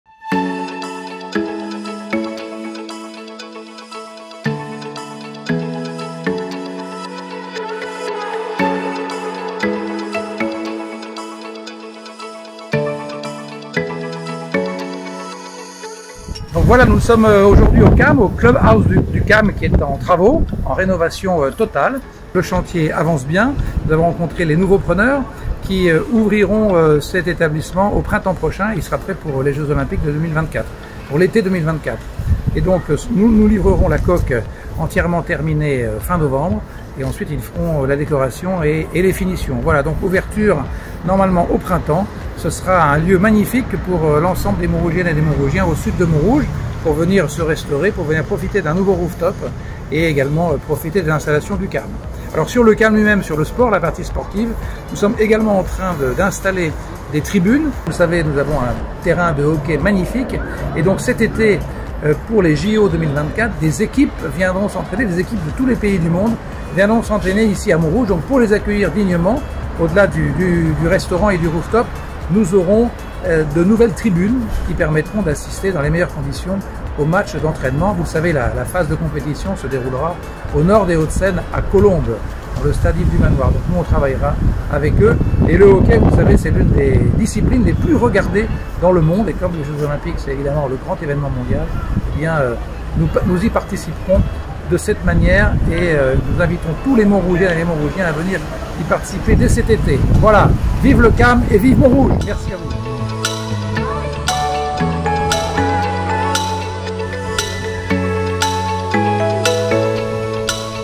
Étienne Lengereau Maire de Montrouge fait le point sur les travaux du CAM
Le 20 juin 2023 s'est déroulée une rencontre entre les différents acteurs de la rénovation du club house du Cercle Athlétique de Montrouge.